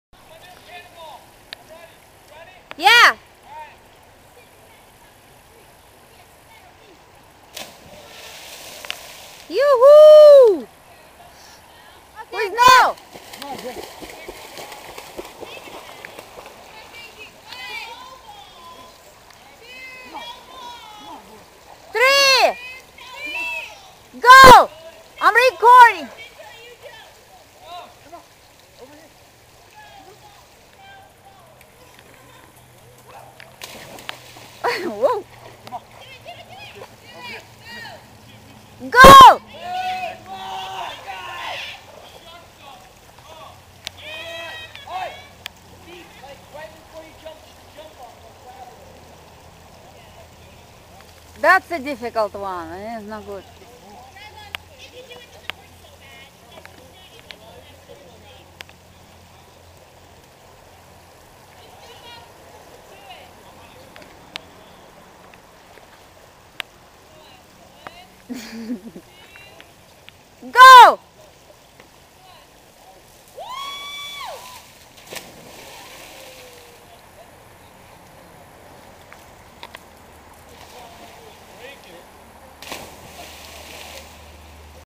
DJ at the Wateringhole
Dj swinging off the Y tree at the wateringhole in Plaff, NPR!